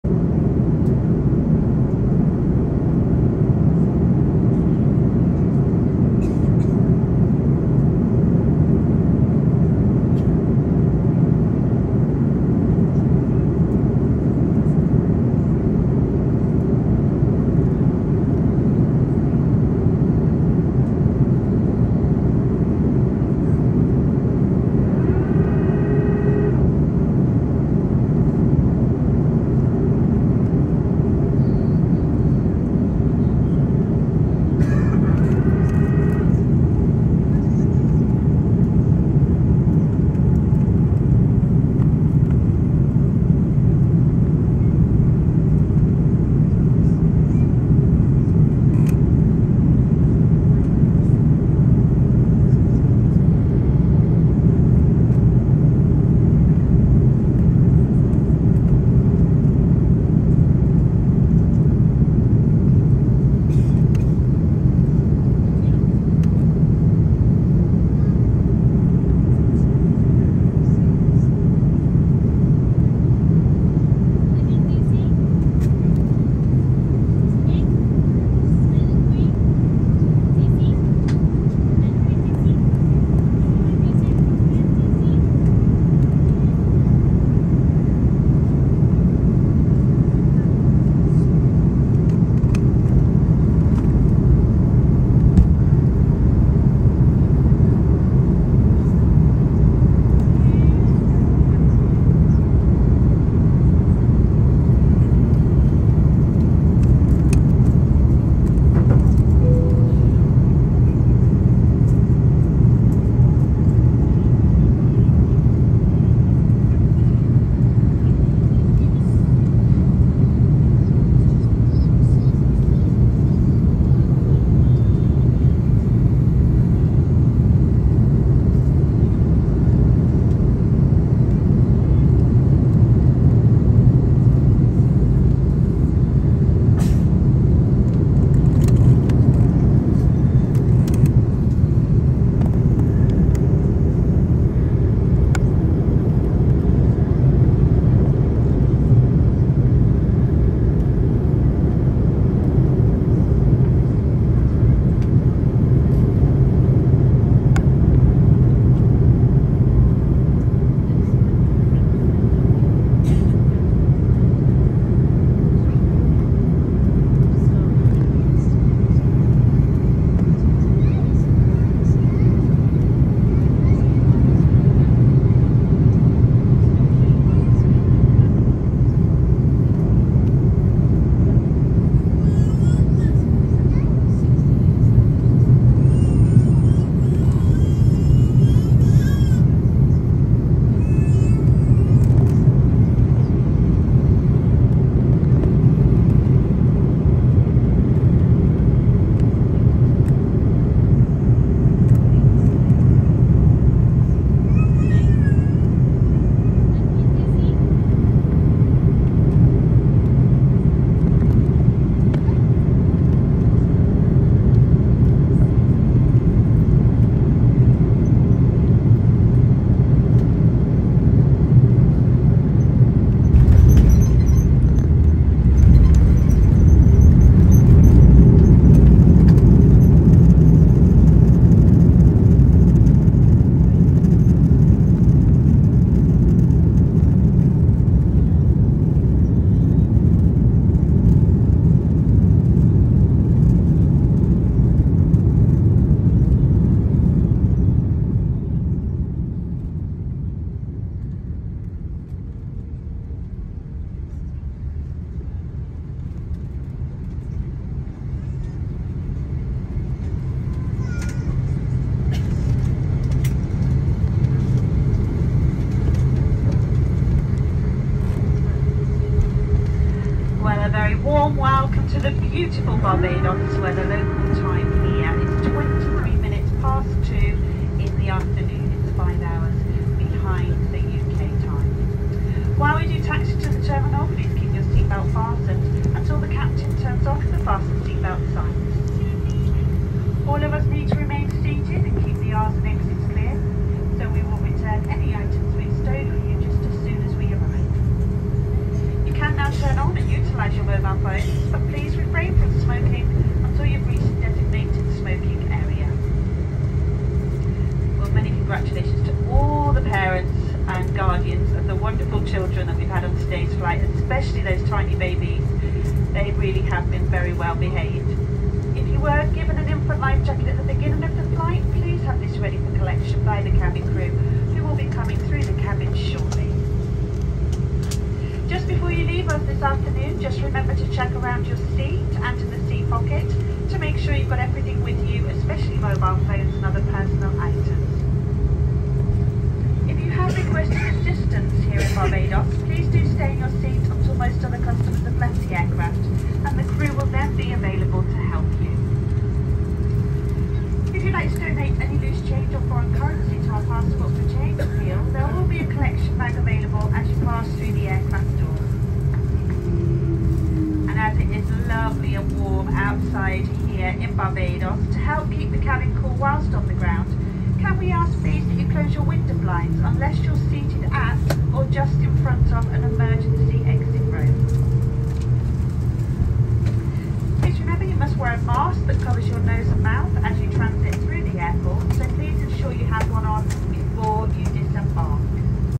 Views of the Barbados coastline from Virgin Atlantic flight VS0177 from London.
Flying from London Heathrow Airport to Barbados on Virgin Atlantic flight VS0177 on June 23rd, 2022.  Take in views of the beautiful coastline with the capital city of Bridgetown and Carlisle Bay before landing at Grantley Adams International Airport (GAIA) (IATA: BGI, ICAO: TBPB).